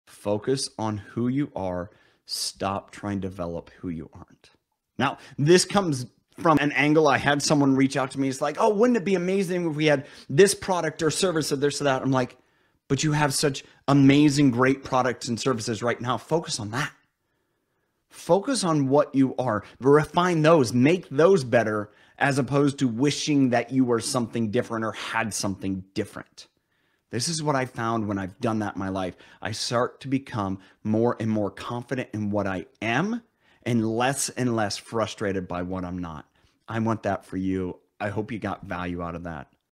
This is Why You Just Need to Focus on Who You Are
Thought of the day 178//

Focus on who you are; stop trying to develop who you aren't.

Now, this comes from an angle. He's like, "Oh, wouldn't it be amazing if we had this product or service, or this, or that?" I'm like, "But you have such amazing, great products and services right now. Focus on that." Focus on what you are. Refine those, make those better, as opposed to wishing that you were something different or had something different.

This is what I've found when I've done that in my life. I start to become more and more confident in what I am and less and less frustrated by what I'm not. I want that for you. I hope you got value out of that.